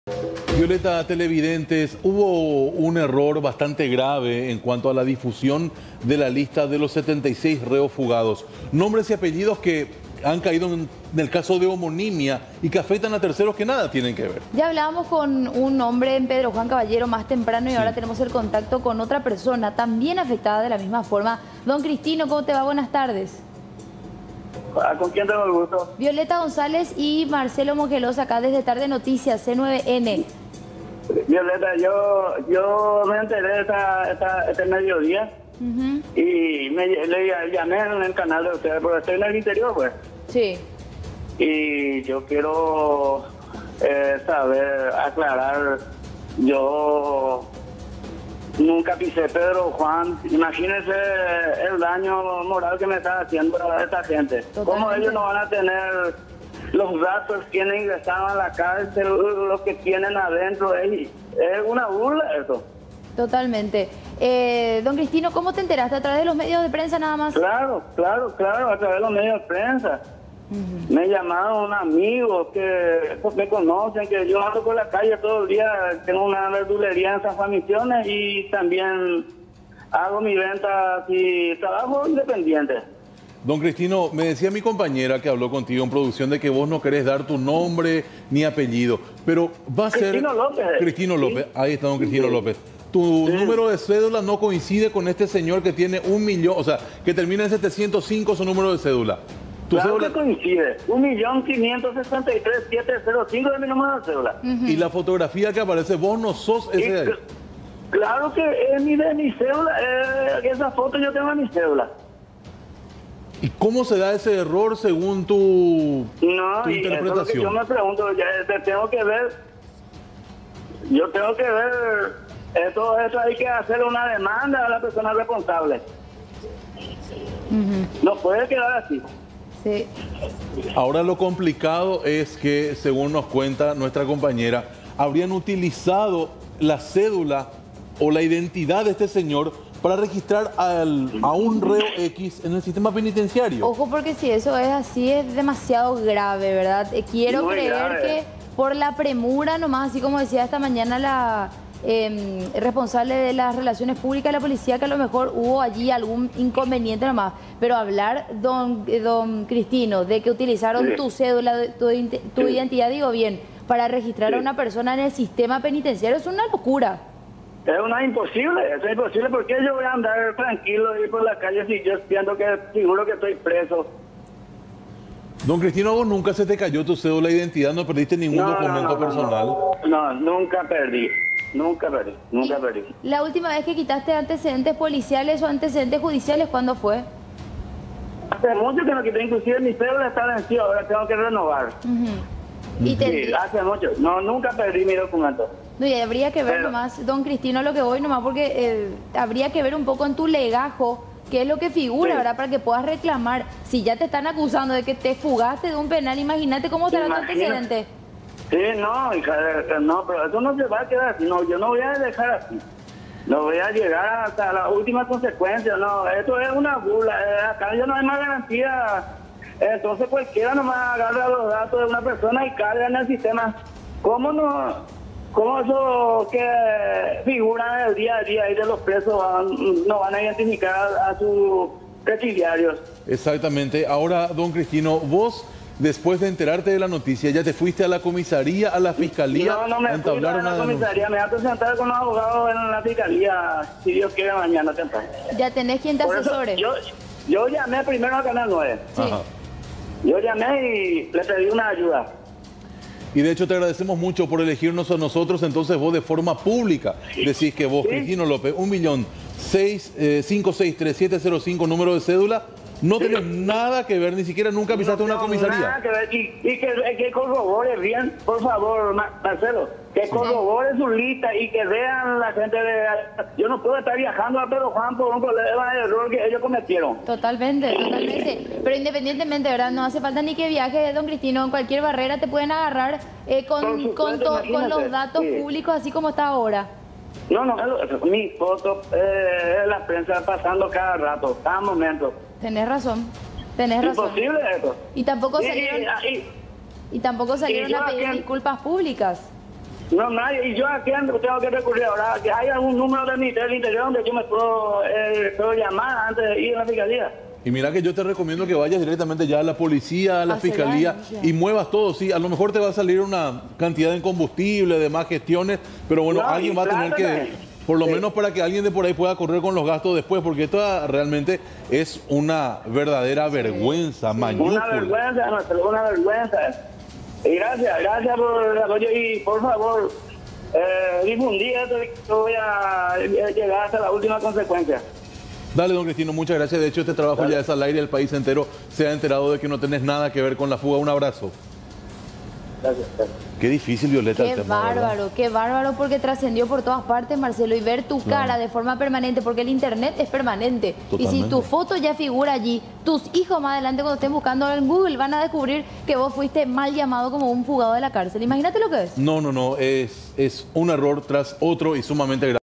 Suman afectados por error en la lista de reclusos fugados de PJC
En las últimas horas, la publicación de los 76 reclusos fugados del penal Pedro Juan Caballero ha causa conmoción en la ciudadanía. Varias personas han sido involucradas en la lista por error. Conversamos vía telefónica con otro de los apuntados erróneamente en la lista